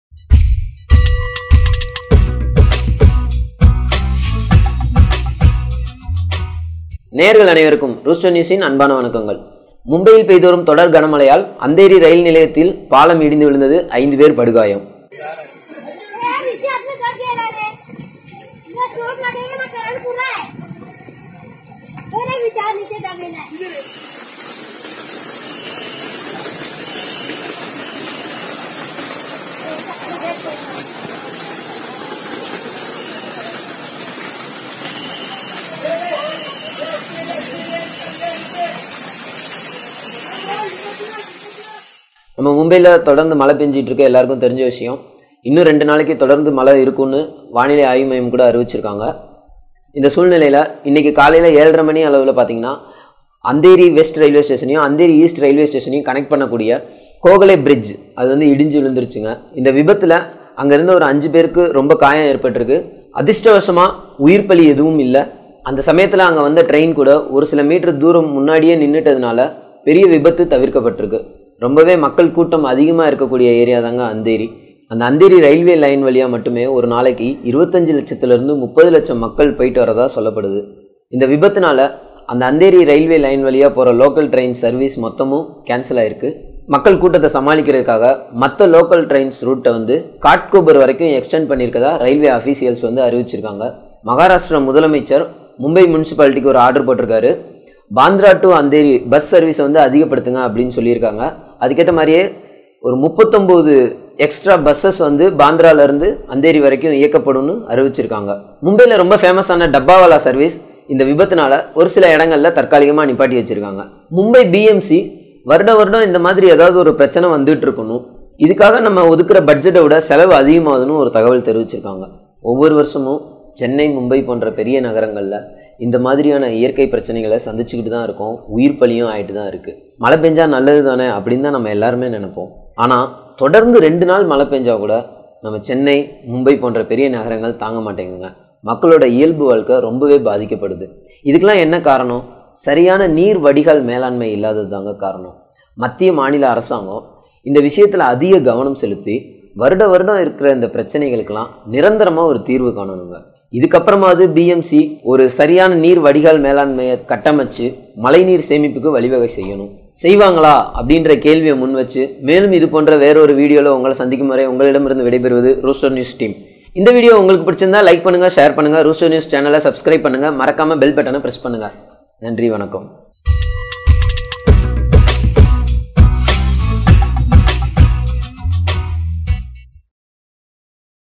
மும்பையில் கனமழை 5 பேர் படுகாயம் | RAIN IN MUMBAI | THE ROOSTER NEWS
mong the 6 injured in the bridge collapse, 2 are in critical condition
Gokhale bridge in Andheri station crashed at around 7.30 am
The bridge connects Andheri East and Andheri West stations.The routes of some local trains on other lines have been extended to clear the spillover at Ghatkopar station, officials said. Local trains are running between Churchgate (in south Mumbai) and Bandra (in the western suburbs), and between Virar (located in neighbouring Palghar district) and Goregaon (western suburb), a Railways official told news agency Press Trust of India.Operations at the Harbour line section have been restored. The first local train ran at 2.19 pm, since the bridge collapse occurred. Railways minister Piyush Goyal had earlier tweeted saying railway officials were working towards restoring harbour line operations by 2 pm.

Trains running from the Harbour line section run from Andheri station to Chhatrapati Shivaji Maharaj Terminus (CSMT) in south Mumbai.

The bridge crash damaged a portion of the platform roof, blocked railway lines and damaged high-tension electric wires and overhead power equipment, according to an IANS report. A team of engineers are working to restore services, an official said.